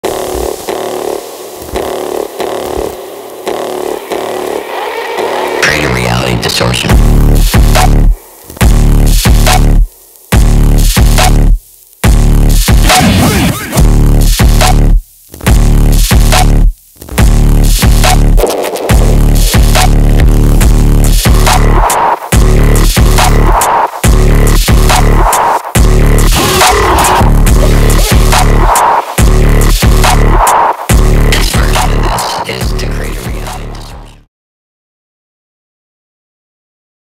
Forbidden Wear x Reality Distortion collection online
Limited Edition of new Forbidden Wear collection is now online.
Tees, Hoodies, Sweatpants, Snapbacks. No Reprint.
Quality screen print on eco friendly textile with high G/M.
Each purchase get a exclusive card with QR code for full Forbidden Society Recordings discography download.
Free stickets included.